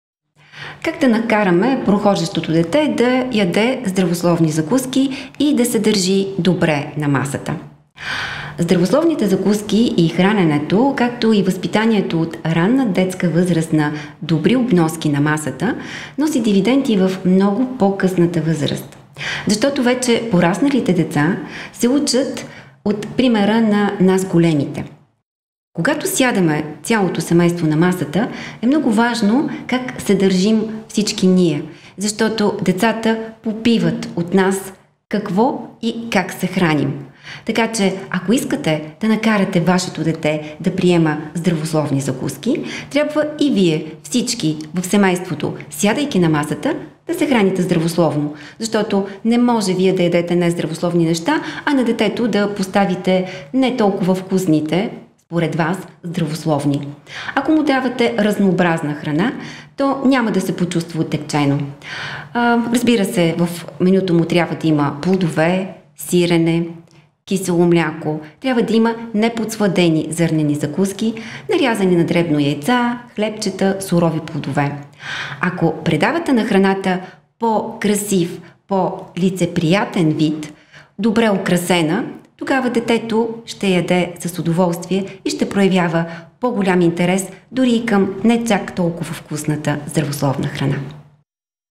Как да накараме детето да се храни правилно
Онлайн училище за родители - Dechko.bg, ви представя лекцията на д-р Антоанета Антонова, в която тя ни предлага различни методи да приучим детето ни да се храни правилно.